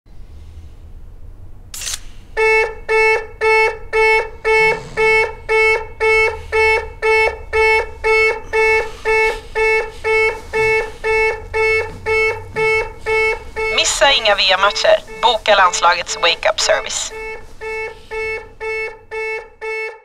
Oddset Dam VM Fridolina Rolfö 20s
Missa inga VM-matcher, boka Oddset och Svenska landslagets Wake-up Service.
VM på andra sidan jorden är snart igång. Beställ väckning för att
 komma upp i tid och heja fram vårt Svenska landslag till vinst. Det är
 Kosovare Asllani och Fridolina Rolfö som ringer och väcker dig inför varje match.